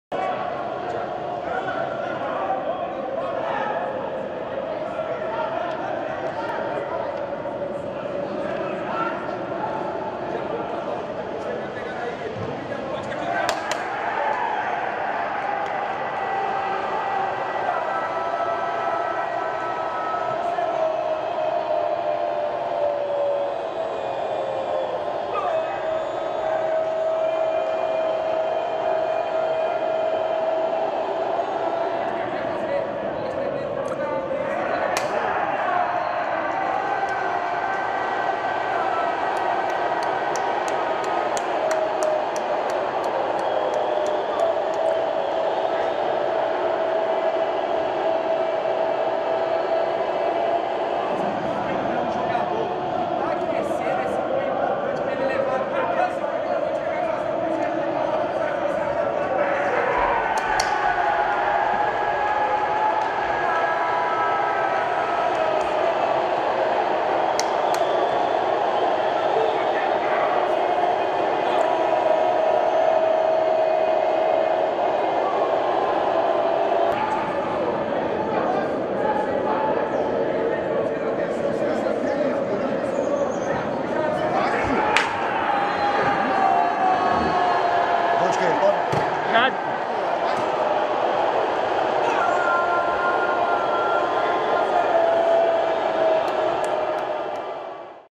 GOLS - CRUZEIRO 4 X O A. PATROCINENSE
A Agência Espacial Comunicação envia conteúdo para diversas agências de noticias espalhadas por todo Brasil desde 2006. Conteúdo jornalístico e esportivo. No último domingo dia 25/04/2021 enviou os gols da partida entre Cruzeiro E.C x Alético Patrocinense para os portais de notícia da DAZN.